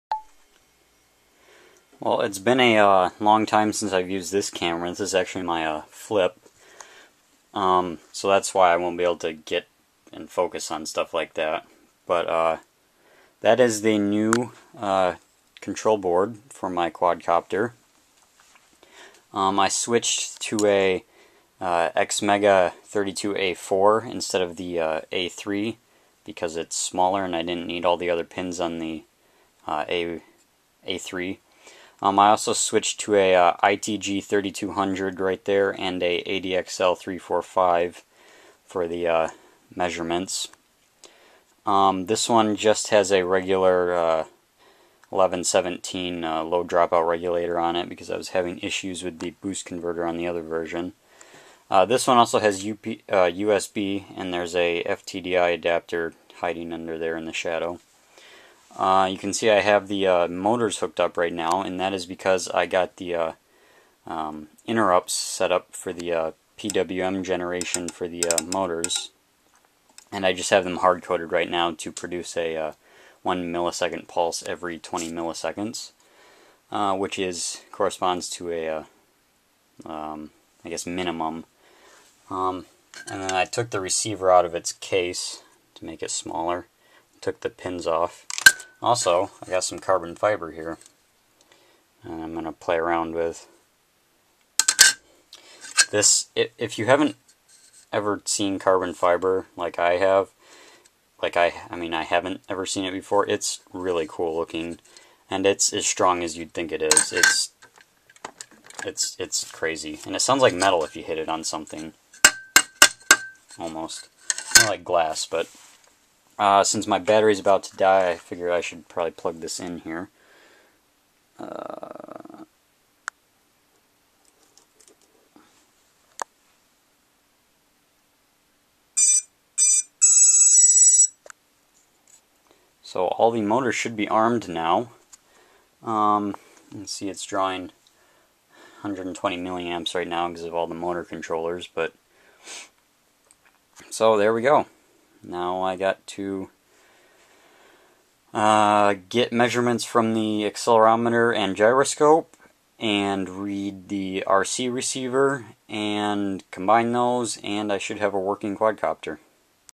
tinyCopter Update - Motor Control
I got my revision C boards and am (finally) starting work on the code. My first order of business was to get motor control working, and with the Xmega TC_driver library, it was easy.